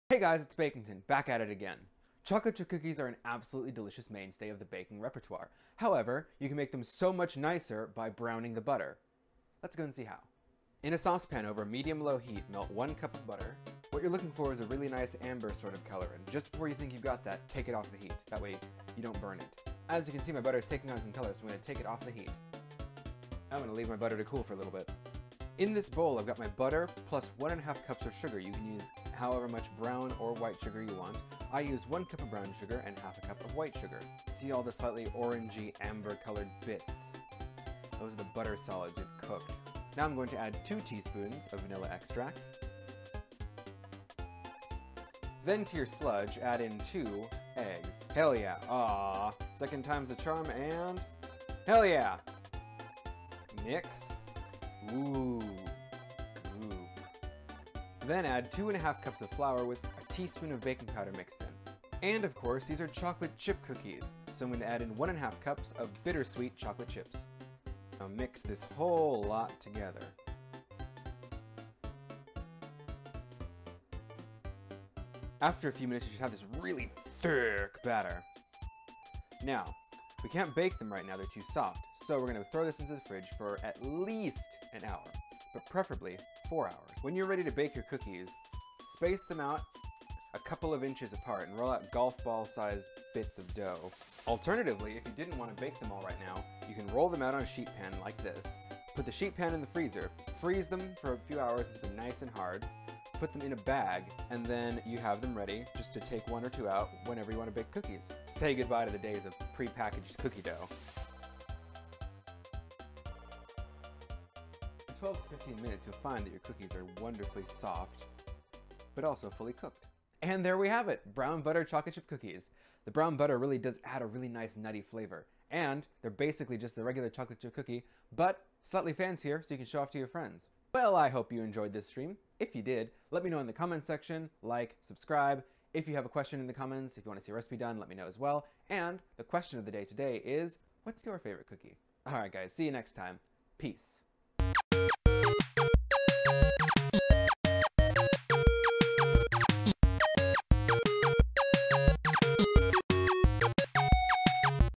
Brown Butter Chocolate Chip Cookies! | Bakington's Bites
Brown Butter Chocolate Chip Cookies are the tier 2 upgrade from the standard cookie! With a nutty flavor, soft texture, fancy sounding name, with minimal difficulty, these are great for both new and seasoned bakers alike! Just be sure to keep an eye on the butter as you brown it!